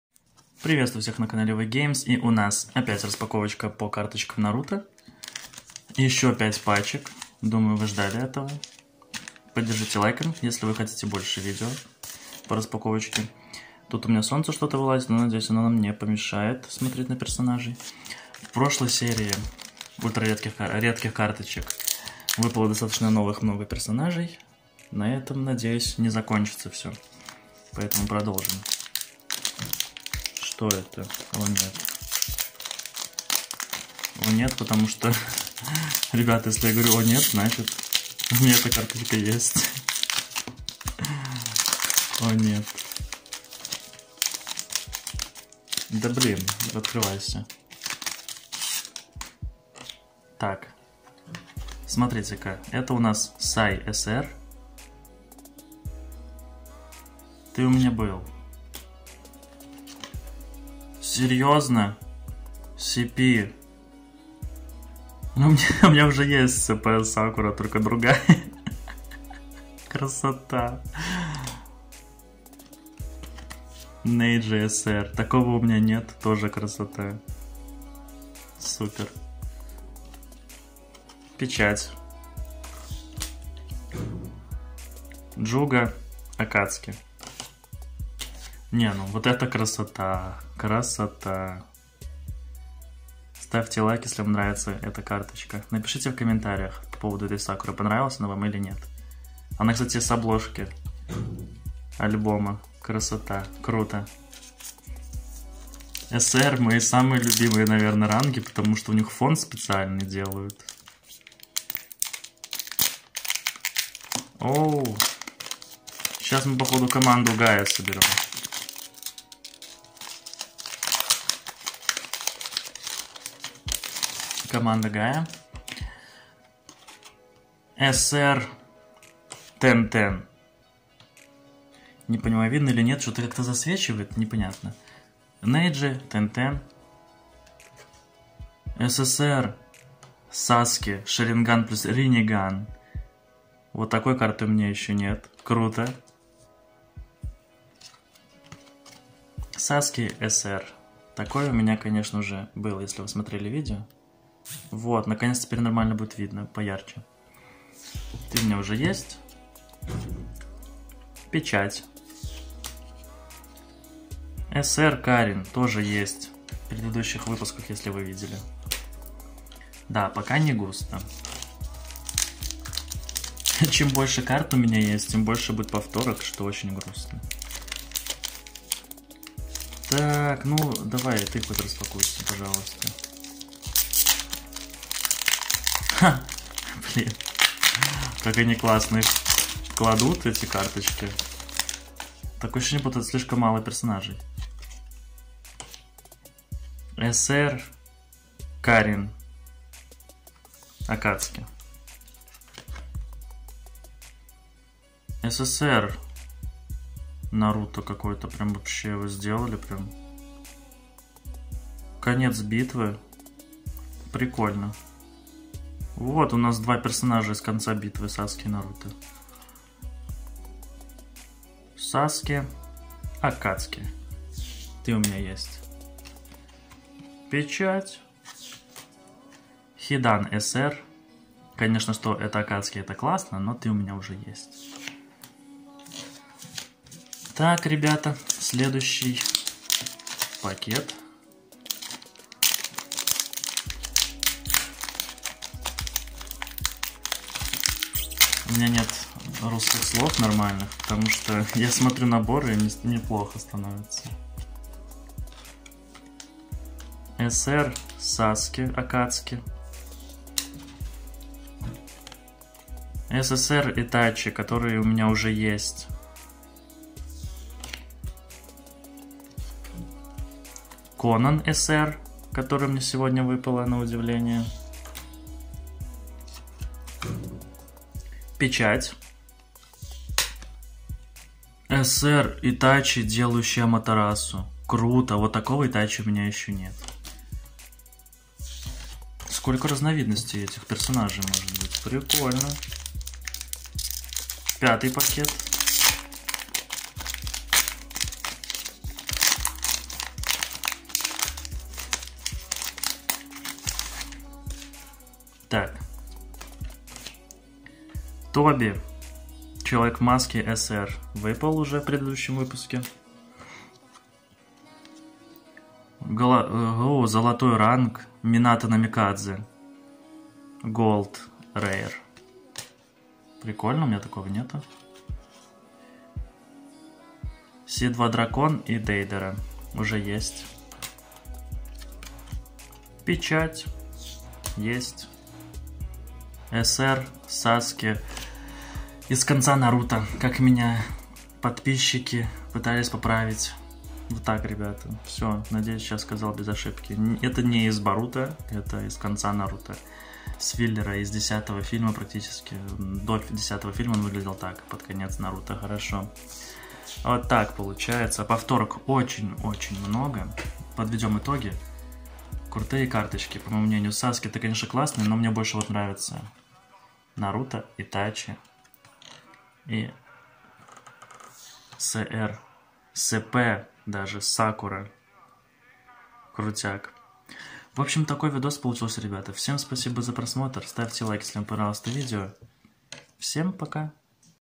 Супер Редкие Карточки Naruto #2 Выпали SR Itachi и CP Sakura | Collection TCG cards Naruto
Суперредкие коллекционные карточки, trading cards game, unboxing, CP Sakura, SSR Kisame Hoshigake